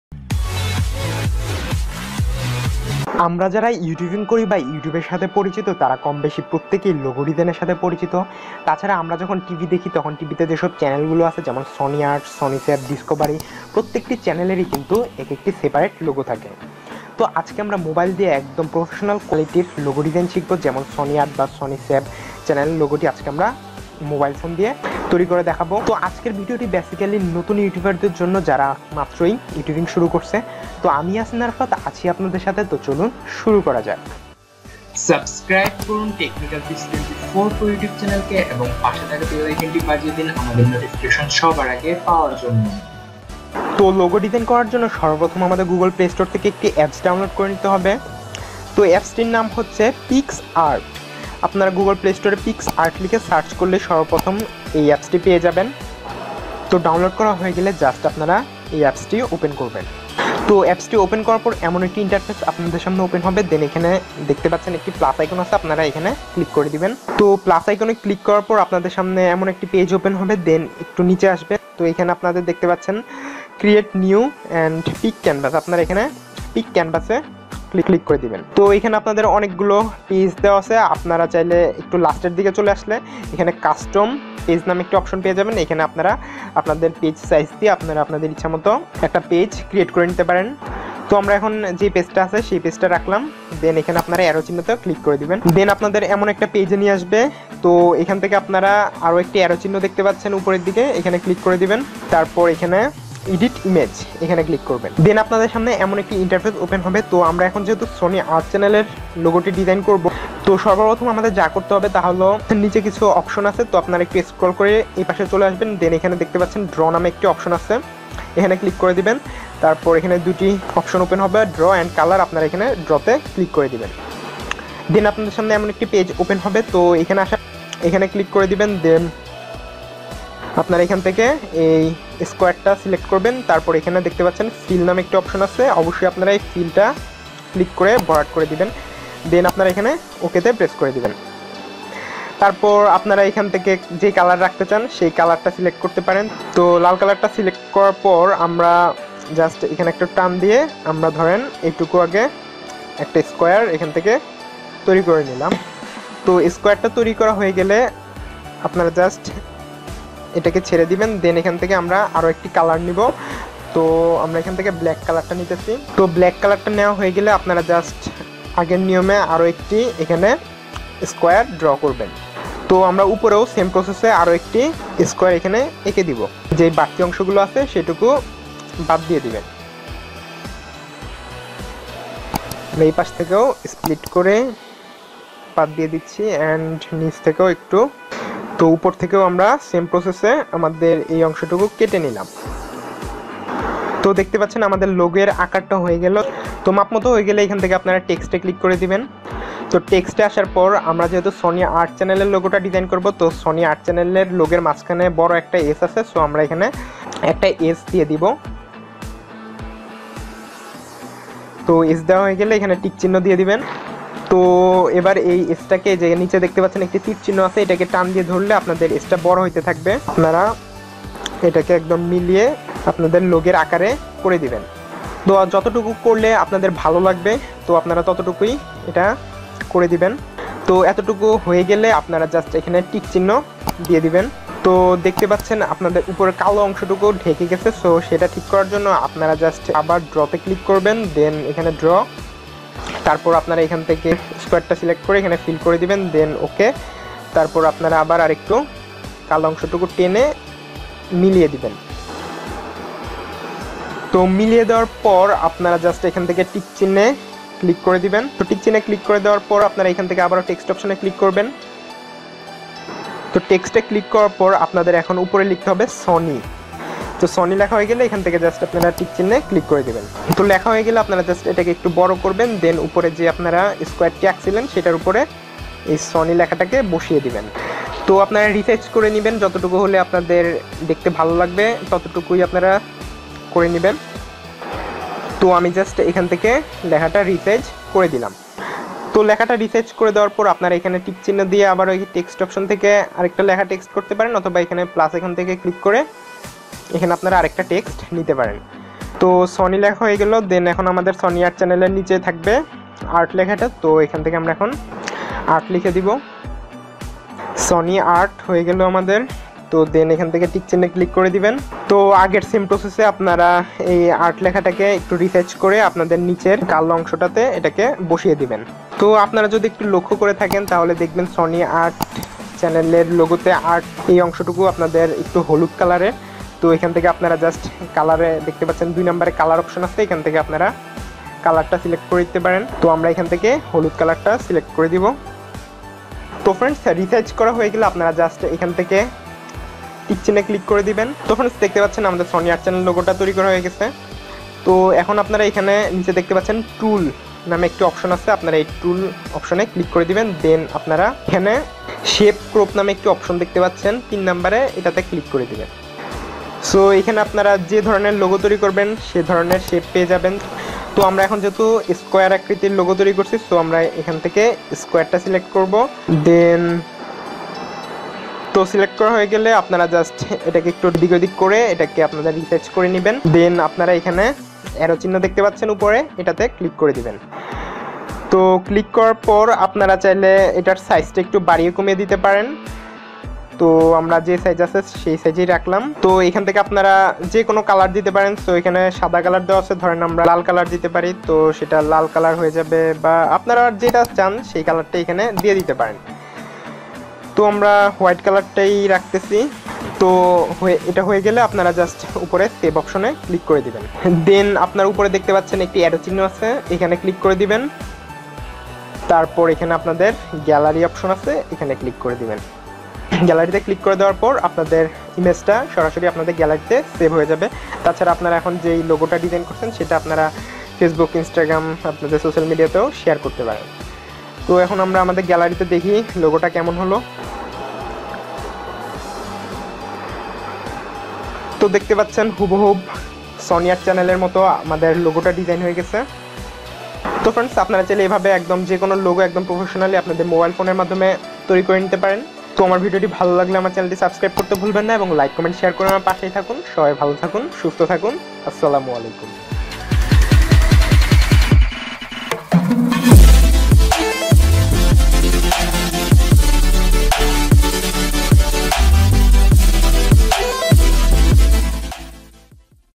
Logo Design Bangla Tutorial🔥| Professional Logo Design 2019 | Mobile Logo Design
In this video I will teach you about Professional Logo Design using mobile phopne...This tips is very easy.